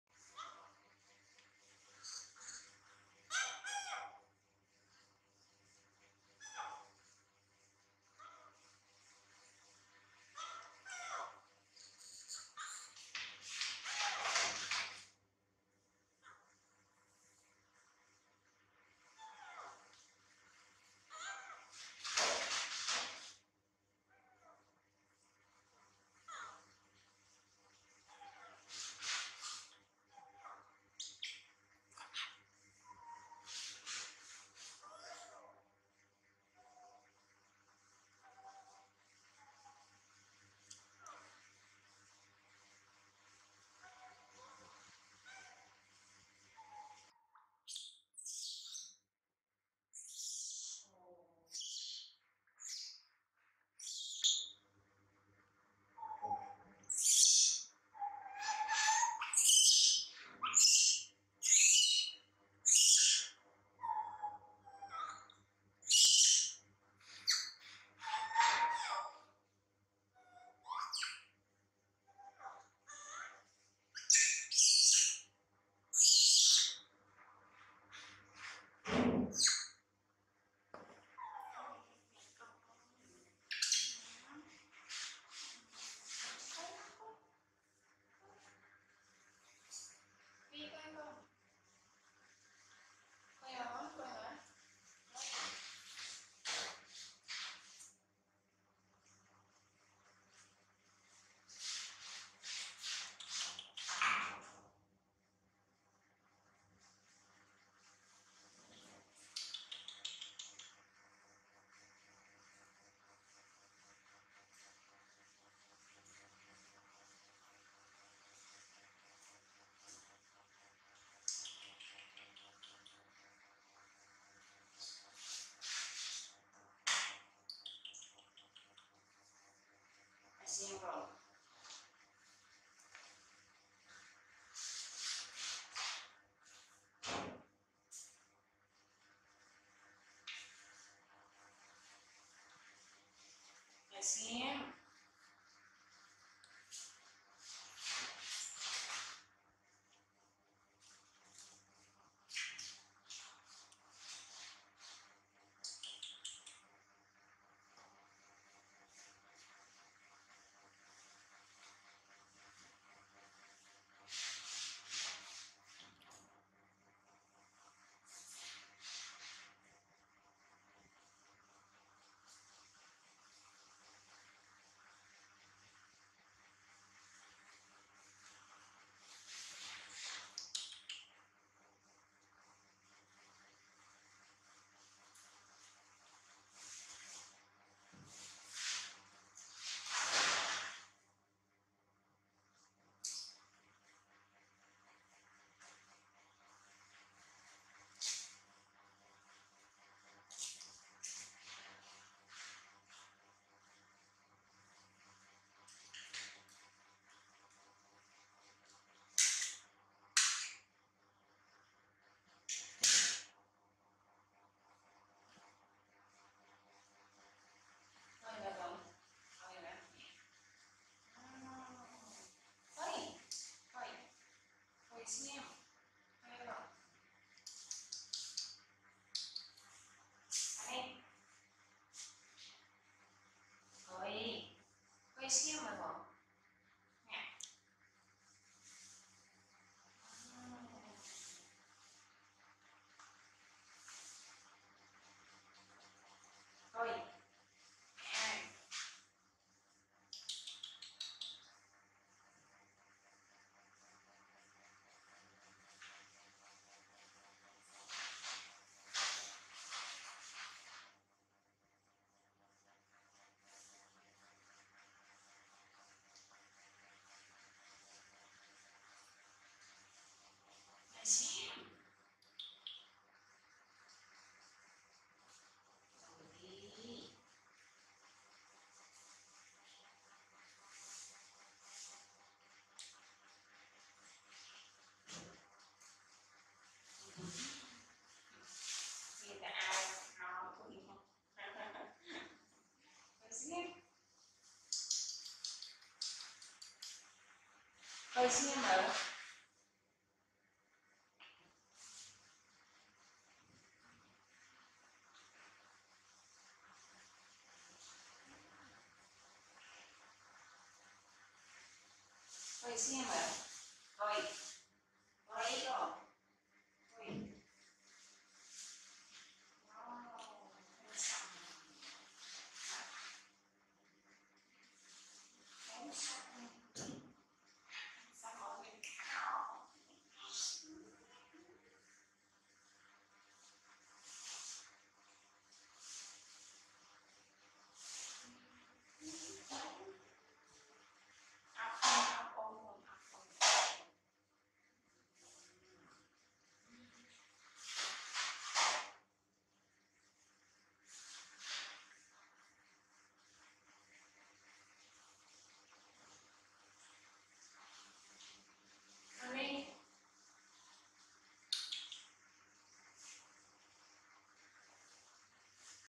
Lil Doddy Cry Tantrum Anqry Mom Late Comforrt & Attention Him
Hello Everyone !!